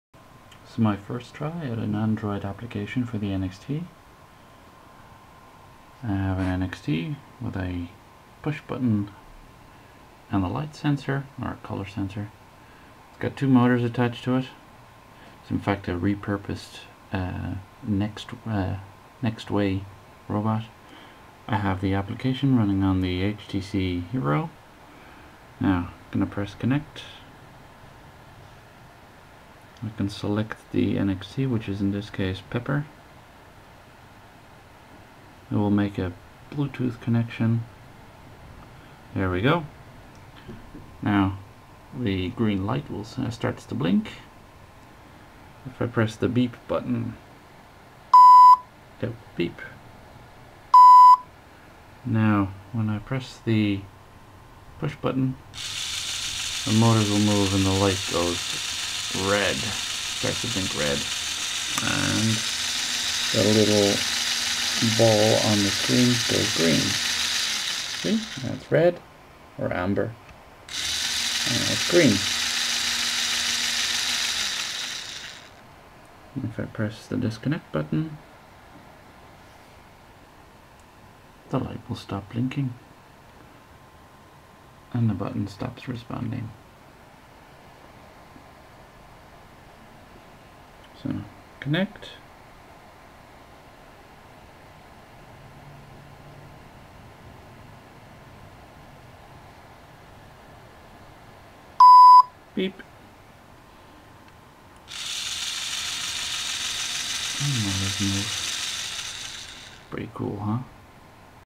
First Android App
Android App Inventor makes programming for the NXT and Android very easy. This was in fact my first program for the Android platform and it took about 1.5 hours or so. Not bad for a windy and wet Sunday afternoon.